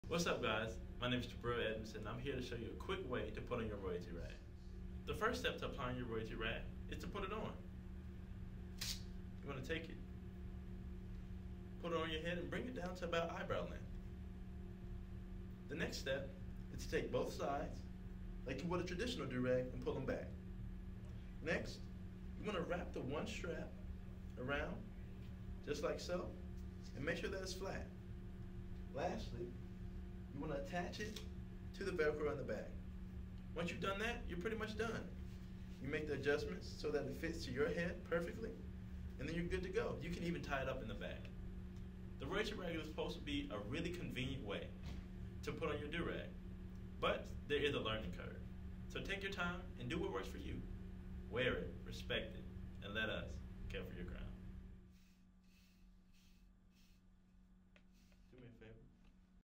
How To Wear the Royalty Rag
Although this is the most efficient durag on the market, it can take some getting used to. Check out this super simple video that walks you through exactly how to wear the best durag you'll ever use.